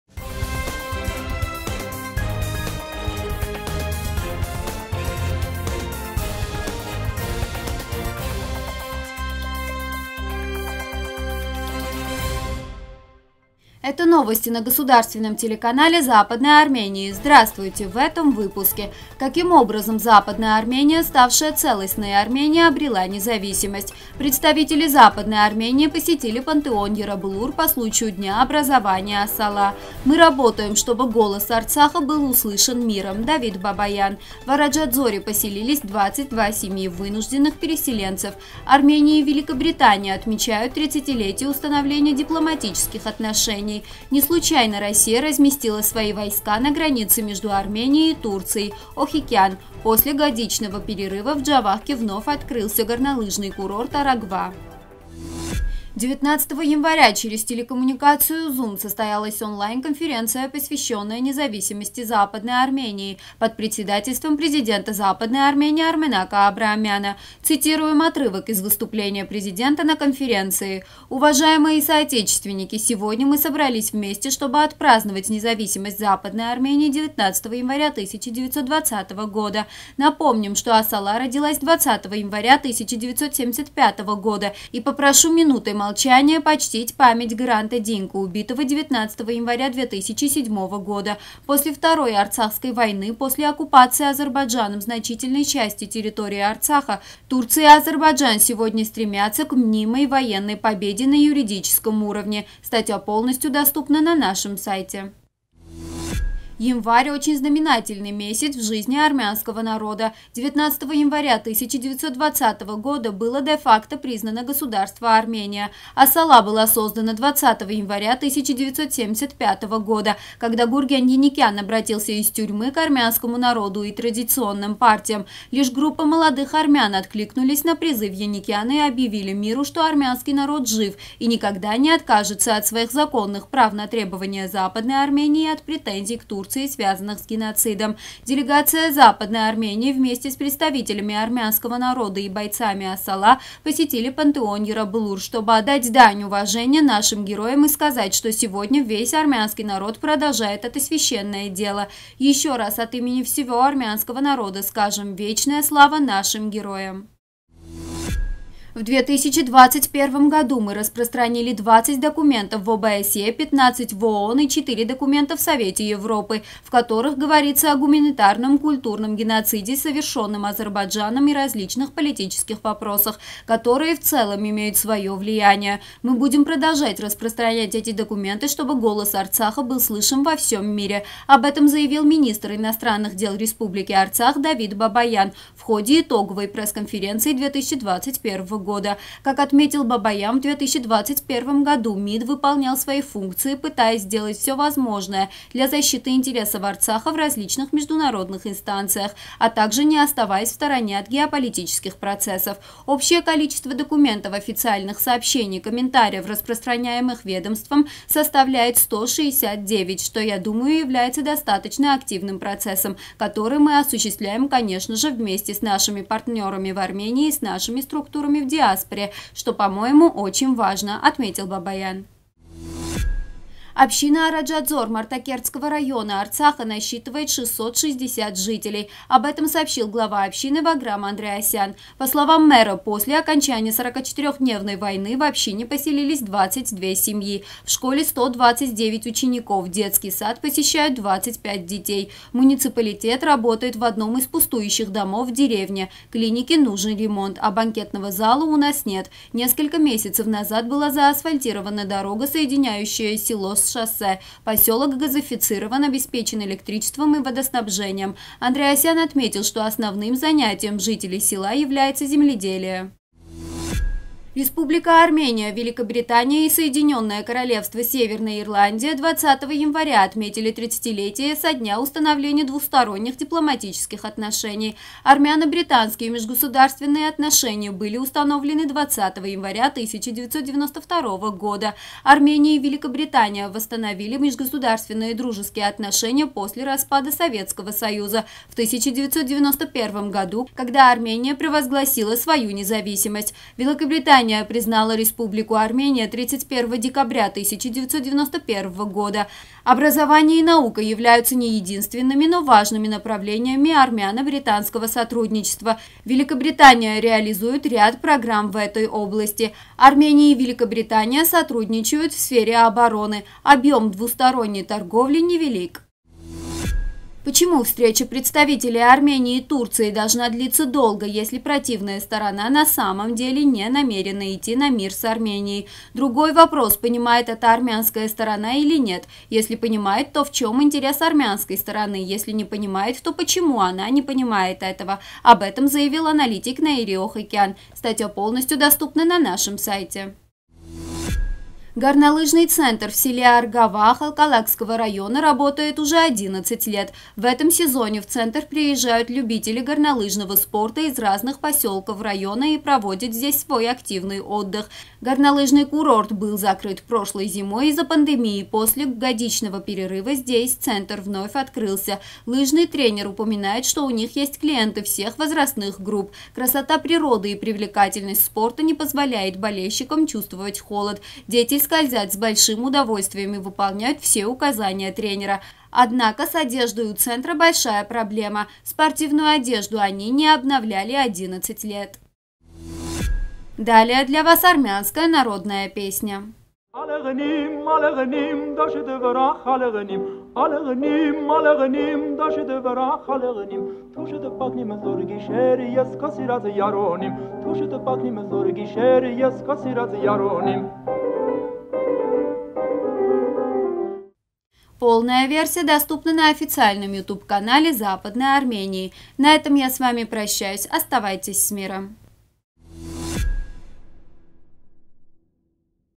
Армения и Великобритания отмечают 30-летие установления дипломатических отношений
1.Каким образом Западная Армения, ставшая Целостной Арменией, обрела независимость?
2.Представители Западной Армении посетили пантеон Ераблур по случаю Дня образования АСАЛА.
3.Мы работаем, чтобы голос Арцаха был услышан миром: Давид Бабаян.
4.В Араджадзоре поселились 22 семьи вынужденных переселенцев.
5.Армения и Великобритания отмечают 30-летие установления дипломатических отношений.
6.Не случайно Россия разместила свои войска на границе между Арменией и Турцией: Охикян.
7.После годичного перерыва в Джавахке вновь открылся горнолыжный курорт Арагва.